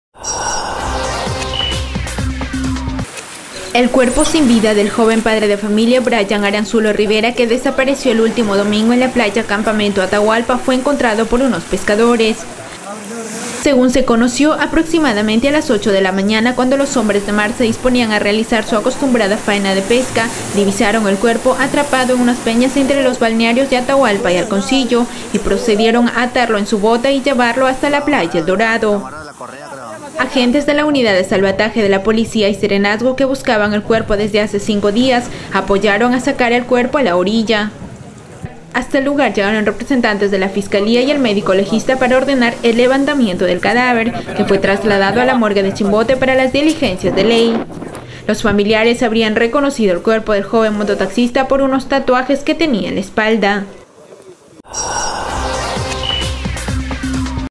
HALLAN CUERPO DE JOVEN MOTOTAXISTA AHOGADO EN PLAYA ATAHUALPA
02 APARECE CUERPO DE AHOGADO.mp4